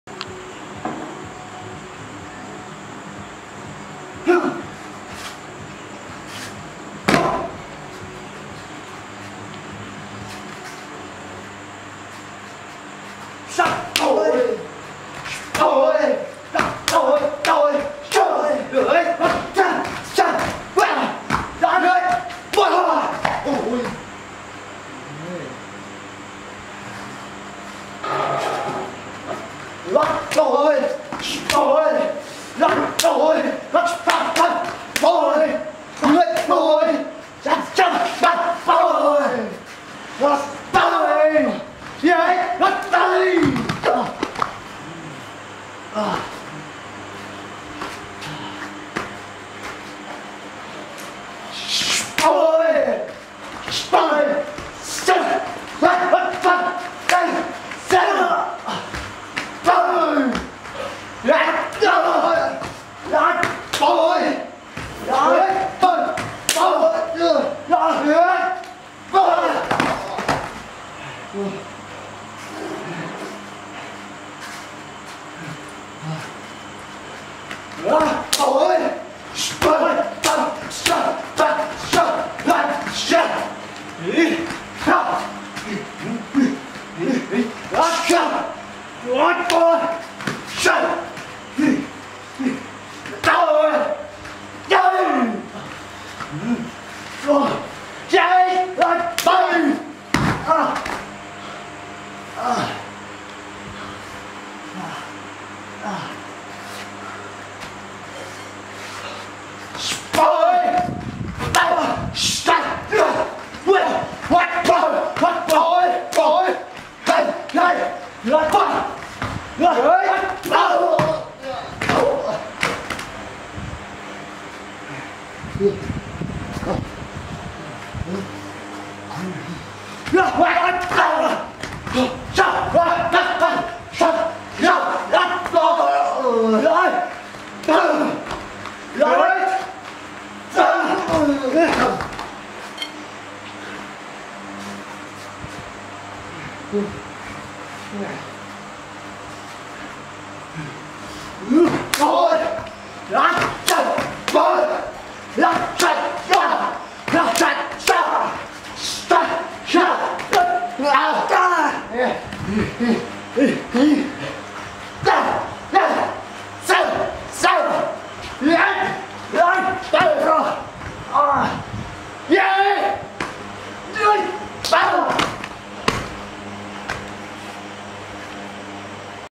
Demonstração de Muay Boran #3
Video curto demonstrando alguns golpes de Muay Boran.
Gostaram do video?

Somente a união irá nos tornar mais forte.
O Muaythai conta com o seu apoio.